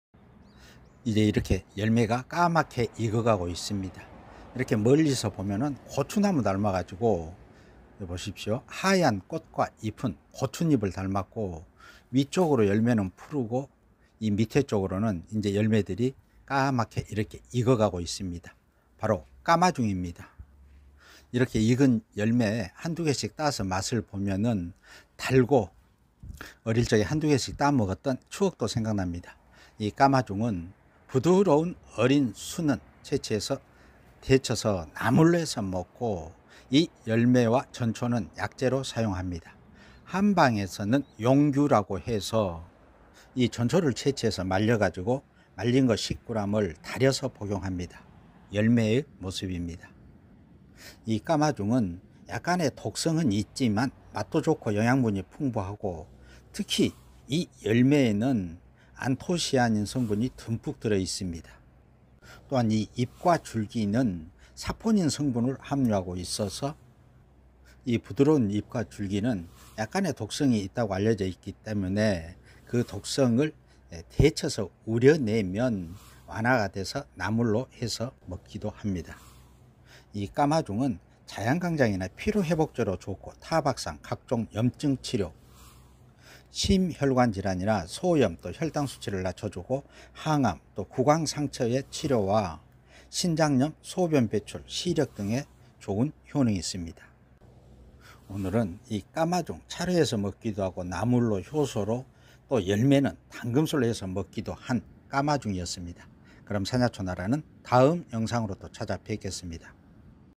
까마중,자양강장,양기를보충한다,나물로,차로드시면,정기를보강하며,염증치료와 암을예방한다~이 약용식물/건강정보 HERBS
자양강장,양기를보충한다~나물로,차로드시면~정기를보강하며,염증치료와 암을예방한다~이 약용식물?